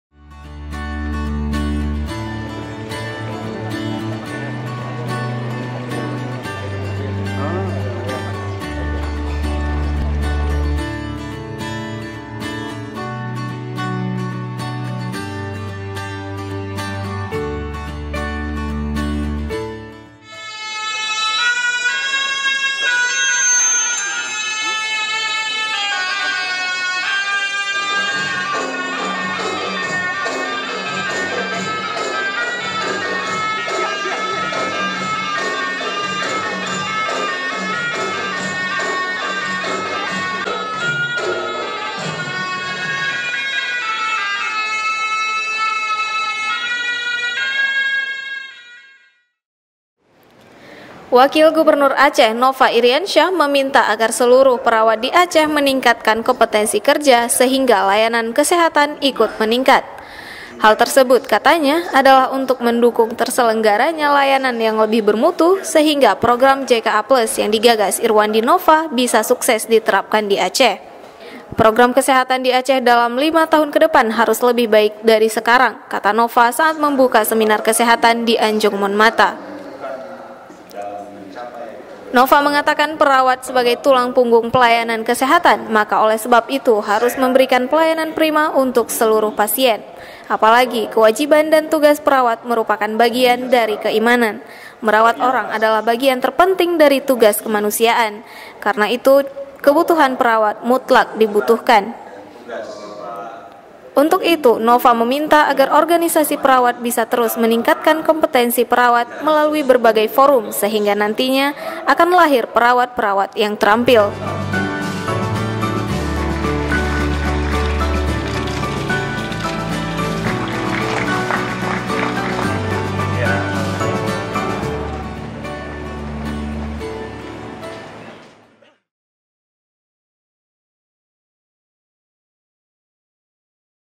Wagub Minta Perawat Tingkatkan Kompetensi Kerja
Banda Aceh — Wakil Gubernur Aceh, Ir. H. Nova Iriansyah MT., meminta agar seluruh perawat di Aceh meningkatkan kompetensi kerja sehingga layanan kesehatan ikut meningkat. Hal tersebut, katanya, adalah untuk mendukung terselenggaranya layanan yang lebih bermutu sehingga program JKA Plus yang digagas Irwandi-Nova bisa sukses diterapkan di Aceh.

“Program kesehatan di Aceh dalam 5 tahun ke depan harus lebih baik dari sekarang,” kata Nova saat membuka seminar kesehatan di Anjong Mon Mata.

Perawat sebagai tulang punggung pelayanan kesehatan, kata Nova, harus memberikan layanan prima untuk seluruh pasien. Apalagi, kewajiban dan tugas perawat merupakan bagian dari keimanan. “Merawat orang adalah bagian terpenting dari tugas kemanusiaan. Karena itu kebutuhan perawat mutlak dibutuhkan.”

Untuk itu, Nova meminta agar organisasi perawat bisa terus meningkatkan kompetensi perawat melalui berbagai forum sehingga lahir perawat-perawat yang terampil.